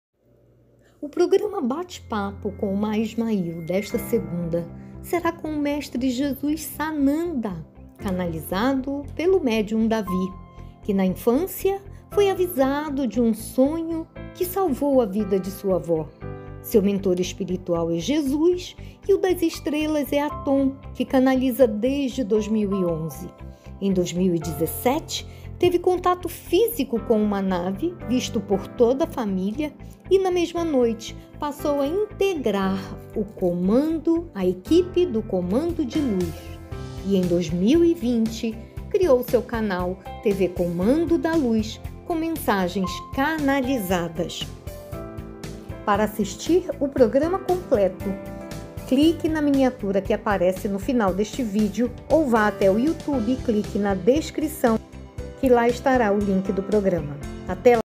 Teaser: Bate-papo com Jesus Sananda - clique na miniatura ou no link na descrição p/ assistir.
Assistam O BATE-PAPO COM UMAIA ISMAIL E SANANDA🙏🙏🙏   Dia 14/11/2022.                O Bate-papo ufológico com Umaia Ismail que ocorre toda segunda 19h fuso Manaus e 20h no resto do país em direção ao sul, e 18h no Acre, no canal @Umaia Ismail do YouTube, e também no Grupo UFOAM CEOANI no facebook e Página CONTATOS EXTRATERRESTRES NA AMAZÔNIA no Facebook simultaneamente, será com o médium David Silva que canaliza seres de Luz e extraterrestres, em especial o Comandante Atom. Algumas vezes, seres como Sananda veem tb, e é ele quem estará conosco nesta segunda.     Sananda seria o nome cósmico de Jesus, pois como sabem, todos os Mestres e profetas dos livros  que ensinam o caminho da Ascensão, estão trabalhando intensamente na atual transição para a elevação planetária, nao só da Terra, mas de todo o Sistema Solar e Galáxia, porém a Terra é o único que, pelo esquecimento de seus habitantes, e maldade dos corações, está demorando e atrapalhando os outros.                       Há 2.000 anos, que significa milésimos de segundos para a eternidade, perigamos ser extintos, e por não sermos de fato, como ocorreu na época dos dinossauros,  devido à interseção de Sananda, inclusive, alguns ridicularizam essa hipótese.           Não se iludam, as relações políticas e sociais entre seres de alta hierarquia e de espécies distintas do Universo é uma norma na eternidade, e só o nível que muda.      A negação disso, e a falta de compreensão de que temos o poder de mudar o curso do planeta é mais uma manifestação do atraso e da amnésia, além de arrogância reinantes aqui.            Quem participa extrafisicamente destas reuniões sabe que elas ocorrem mesmoooooo, e sentirão a verdade destas palavras.  Quem nunca foi numa Assembléia interplanetária ou Galáctica, ou não consegue  assimilar quando volta, devido ao espesso véu de ilusão que cobre a Terra, por favor se abstenha de opinar, pois pode ser motivo de arrependimento quando deixar o corpo e tiver que prestar contas de seus atos e palavras. Dê tempo ao tempo. Enfim... Todos estão convidados a bater um papo com Sananda e depois das perguntas que eu farei, pois o que perguntarei carece de respostas ou merecem confirmação na literatura e nos vídeos atuais, e faz parte de uma pesquisa, abrirei para perguntas.  Compartilhem com todos que estão preparados para essa experiência. E depois analisem com o coração, sem precisar confirmar ou negar mentalmente. Vou trabalhar para que outros canais que acessam essas frequências venham ao canal, para que vocês possam ter mais elementos comparativos. 😉🖖👏🙏🙌 Fiquem na Paz e Bem- Vindos à Ufologia Multidimensional.👽🙌🙏🤝🖖🫶😉